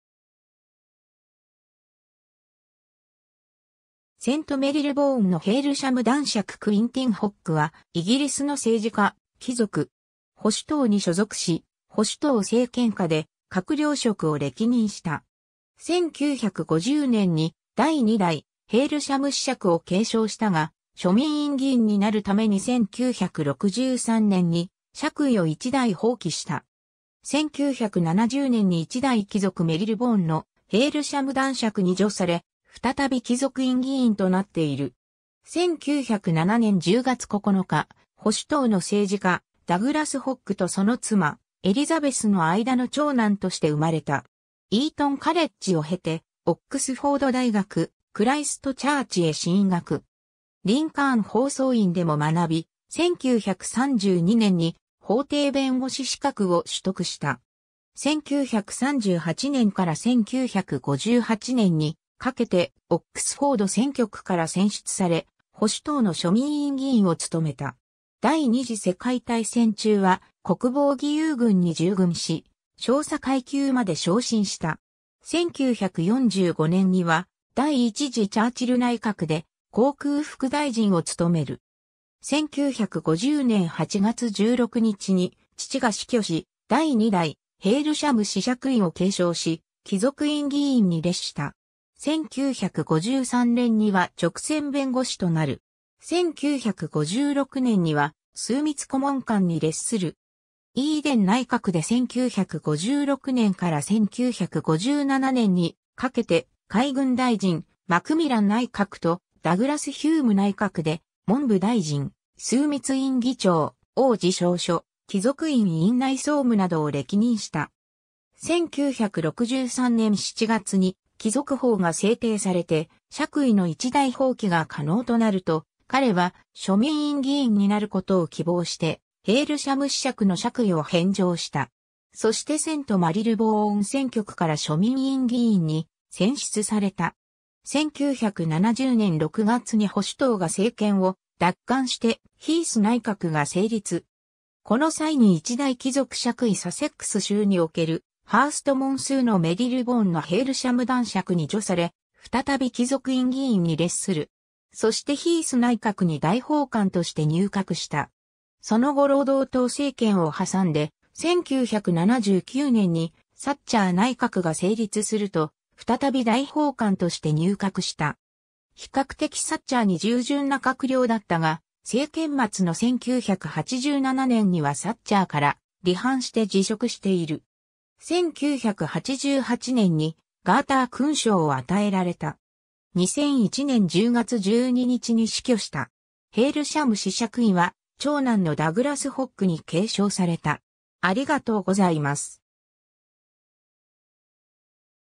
クィンティン・ホッグ
セント・メリルボーンのヘイルシャム男爵クィンティン・ホッグ（英語: Quintin Hogg, Baron Hailsham of St Marylebone, KG, CH, PC, QC, FRS、1907年10月9日 - 2001年10月12日）は、イギリスの政治家、貴族。
 保守党に所属し、保守党政権下で閣僚職を歴任した。
1950年に第2代ヘイルシャム子爵を継承したが、庶民院議員になるために1963年に爵位を一代放棄した。
1970年に一代貴族メリルボーンのヘイルシャム男爵に叙され、再び貴族院議員となっている。
 1907年10月9日、保守党の政治家ダグラス・ホッグ（1929年にヘイルシャム子爵に叙される）とその妻エリザベス（旧姓ブラウン）の間の長男として生まれた。
 イートン・カレッジを経てオックスフォード大学クライスト・チャーチへ進学。
リンカーン法曹院でも学び、1932年に法廷弁護士資格を取得した。
 1938年から1958年にかけてオックスフォード選挙区から選出され、保守党の庶民院議員を務めた。
第二次世界大戦中は国防義勇軍に従軍し、少佐階級まで昇進した。
1945年には第1次チャーチル内閣で航空副大臣を務める。
1950年8月16日に父が死去し、第2代ヘイルシャム子爵位を継承し、貴族院議員に列した。
1953年には勅選弁護士(QC)となる。
1956年には枢密顧問官(PC)に列する。
 イーデン内閣で1956年から1957年にかけて海軍大臣、マクミラン内閣とダグラス＝ヒューム内閣で文部大臣(在職1957年1月-9月、1964年4月-10月)、枢密院議長(在職1957年-1959年、1960年-1964年)、王璽尚書(1959年-1960年)、貴族院院内総務(在職1960年-1963年)などを歴任した。
1963年7月に貴族法が制定されて爵位の一代放棄が可能となると、彼は庶民院議員になることを希望してヘイルシャム子爵の爵位を返上した。
そしてセント・マリルボーン選挙区から庶民院議員に選出された。
 1970年6月に保守党が政権を奪還してヒース内閣が成立。
この際に一代貴族爵位サセックス州におけるハーストモンスーのメリルボーンのヘイルシャム男爵に叙され、再び貴族院議員に列する。
そしてヒース内閣に大法官(貴族院議長)として入閣した。
その後労働党政権を挟んで、1979年にサッチャー内閣が成立すると再び大法官として入閣した。
比較的サッチャーに従順な閣僚だったが、政権末の1987年にはサッチャーから離反して辞職している。
 1988年にガーター勲章を与えられた。
 2001年10月12日に死去した。
ヘイルシャム子爵位は長男のダグラス・ホッグに継承された。